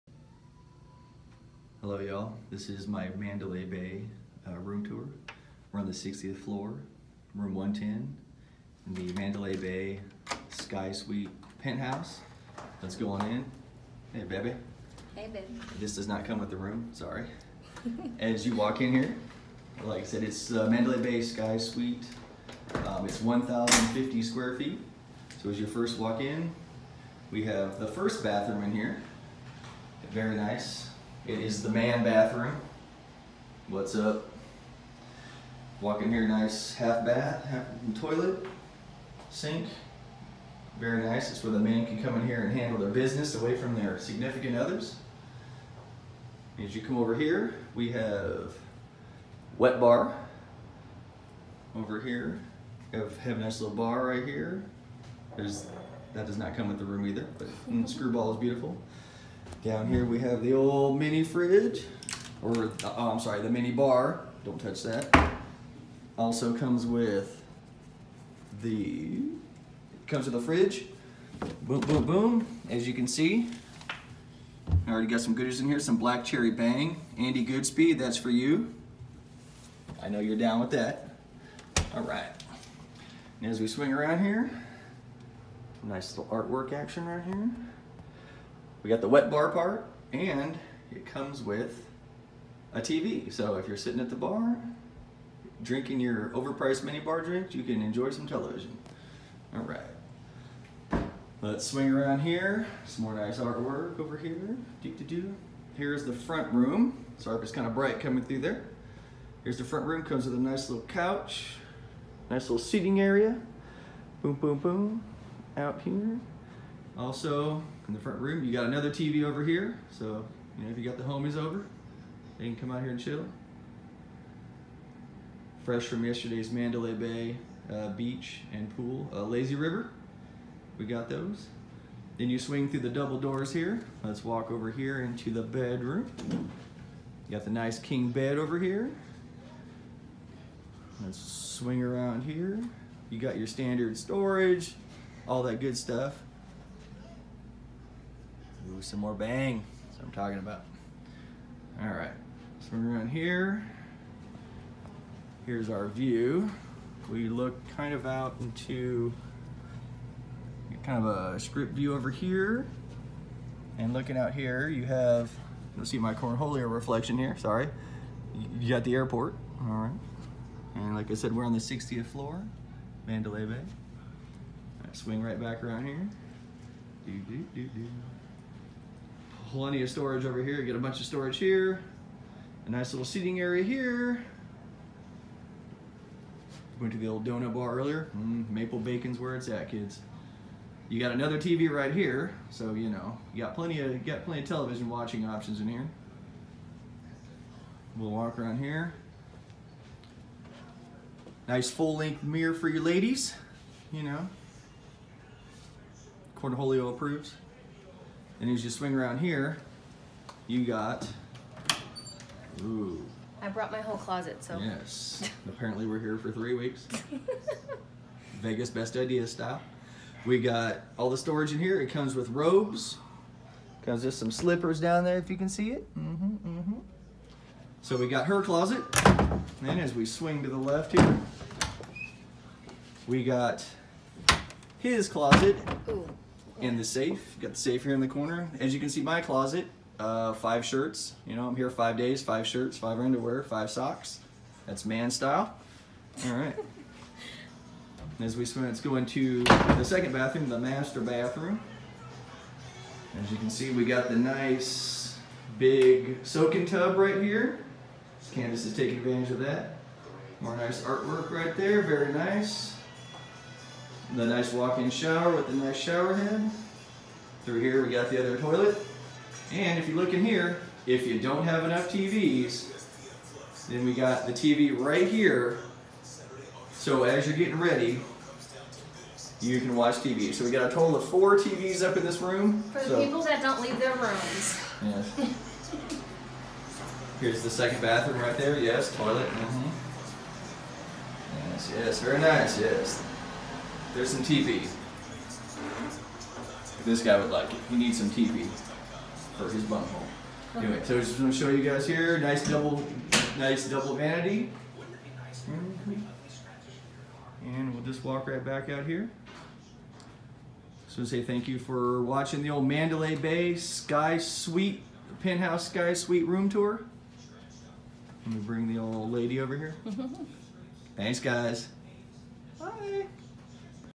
Mandalay Bay Las Vegas One Bedroom Penthouse Sky View Suite Room Tour
Join The Kenos for a Room Tour of our beautiful One Bedroom Penthouse Sky Suite at Mandalay Bay in beautiful Las Vegas!!